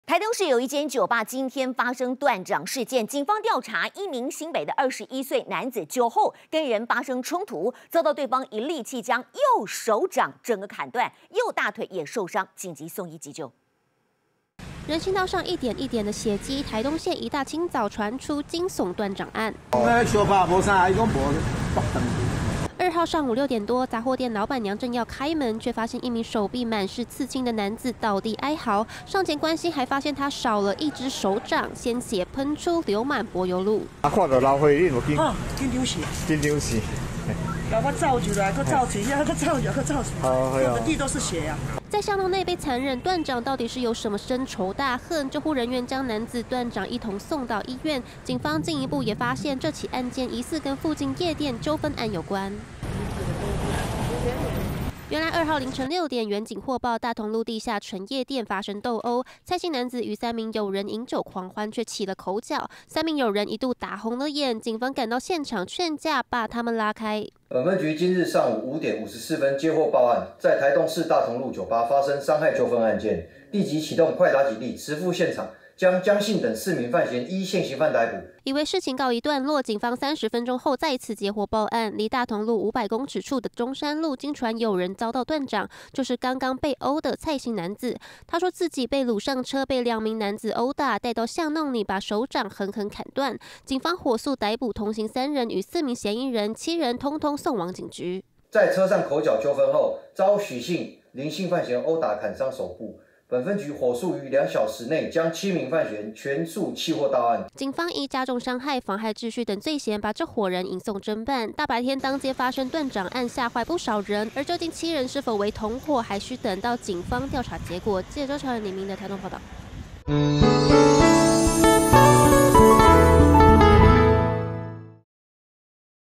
夜店口角！ 男子遭「右手掌遭砍斷」鮮血如注 民眾嚇壞
台東市一間酒吧今（2）日一早發生斷掌事件，警方調查，一名住新北的21歲蔡姓男子酒後與人衝突，遭對方以利器將右手掌整個砍斷，右大腿也受傷，經急救後送醫，目前男子仍在開刀救治中，而警方也依妨害秩序等罪嫌帶回7名犯嫌，調查犯案動機。據了解，當時一家雜貨店老闆娘正要開門，卻發現一名手臂滿是刺青的男子倒地哀嚎，她上前關心，發現男子竟少了一隻手掌，且鮮血流滿柏油路。

下載【年代電視】APP：
iOS平台：